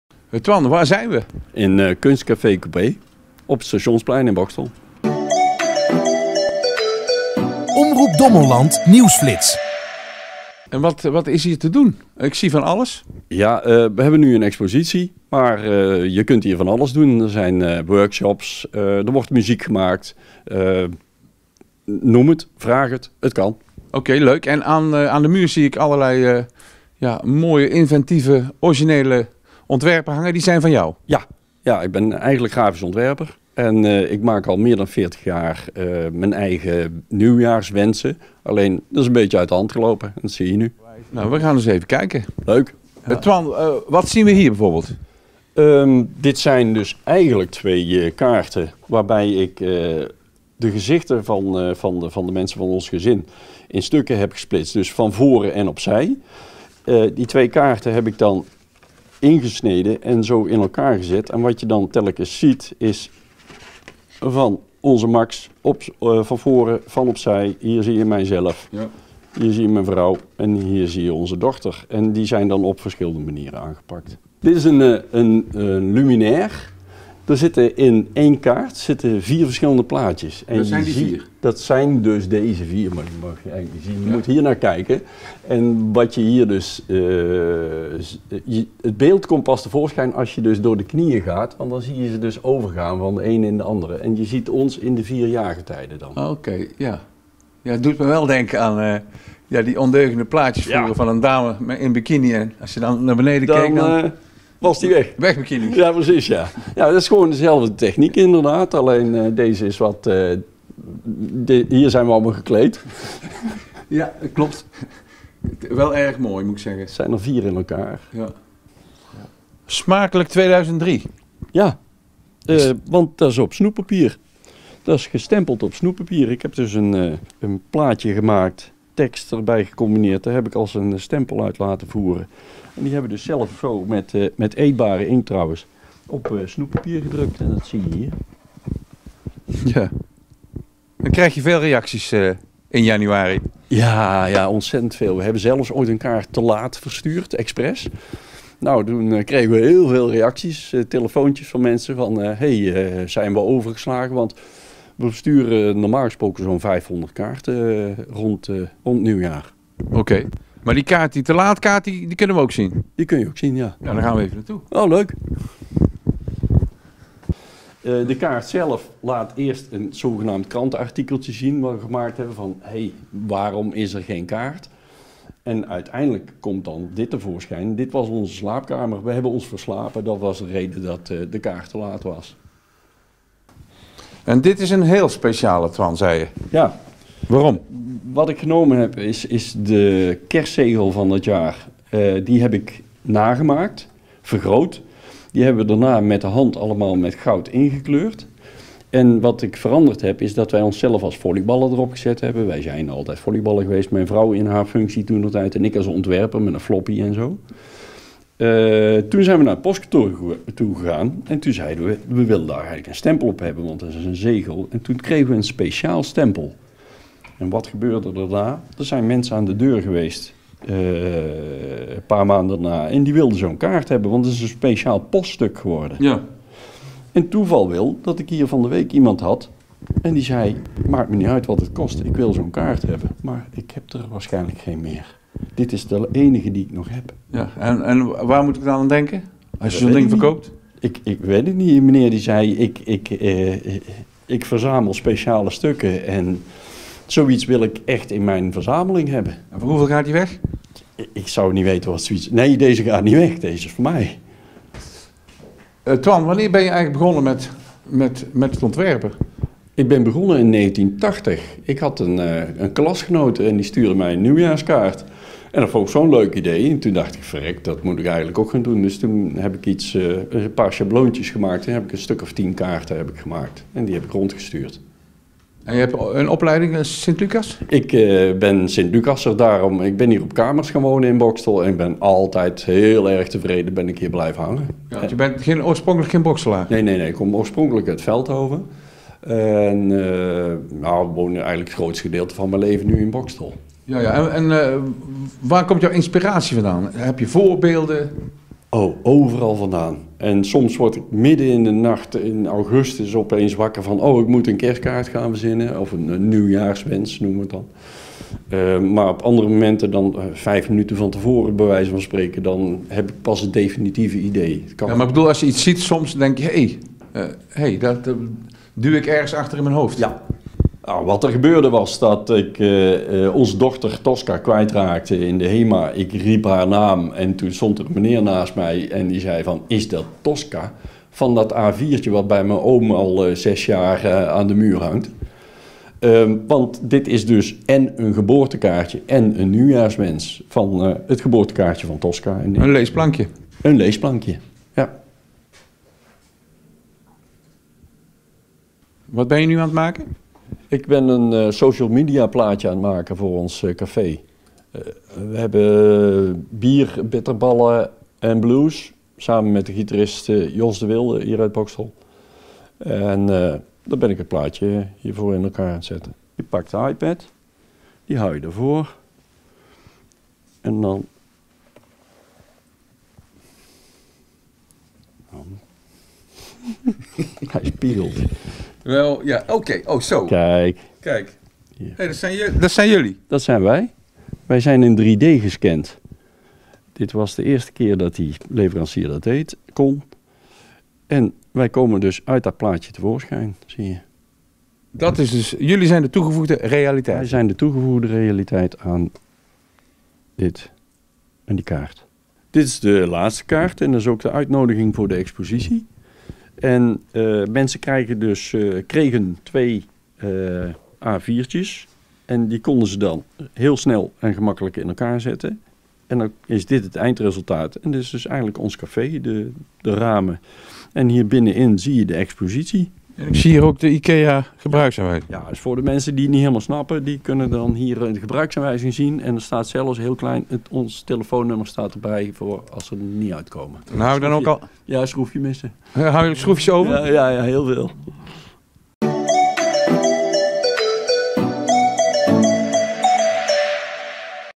KunstCafé Coupé
In hartje Boxtel, tegenover het station, bevindt zich het KunstCafé Coupé. Zeker de moeite om er eens een kijkje te nemen.